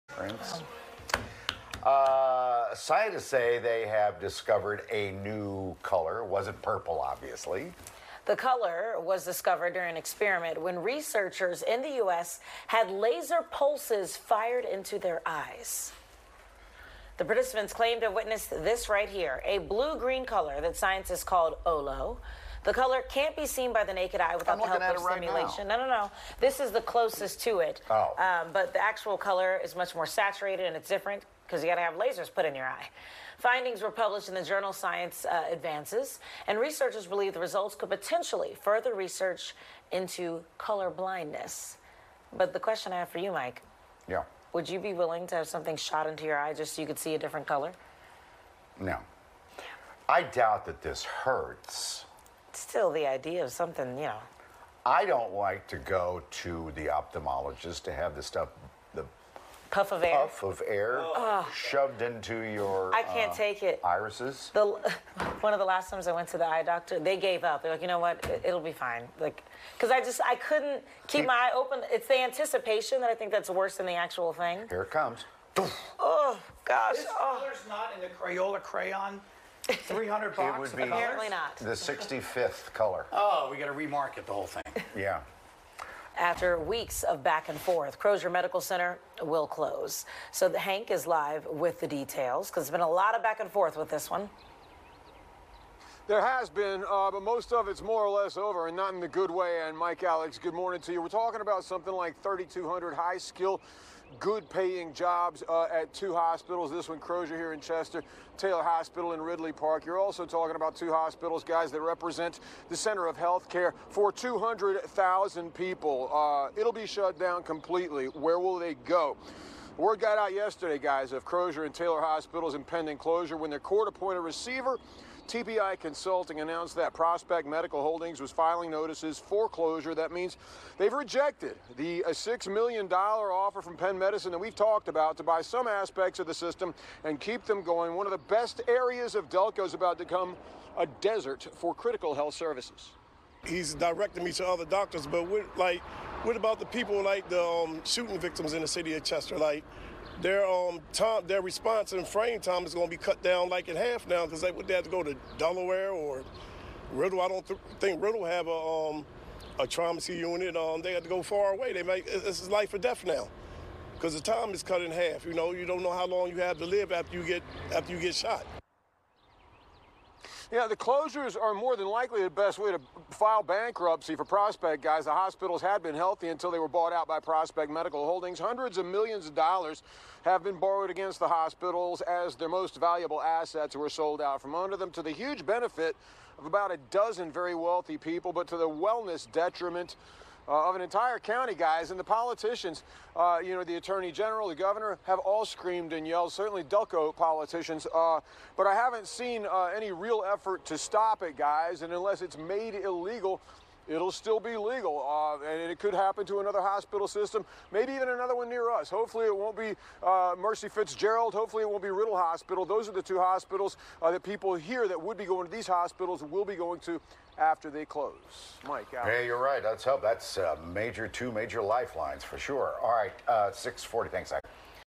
Scientists discover never before seen color
Scientists have discovered a new blue-green color they've named Olo. The new shade is not visible to the naked eye without the help of stimulation. The color included in the video is a close representation, but much more saturated than the real color.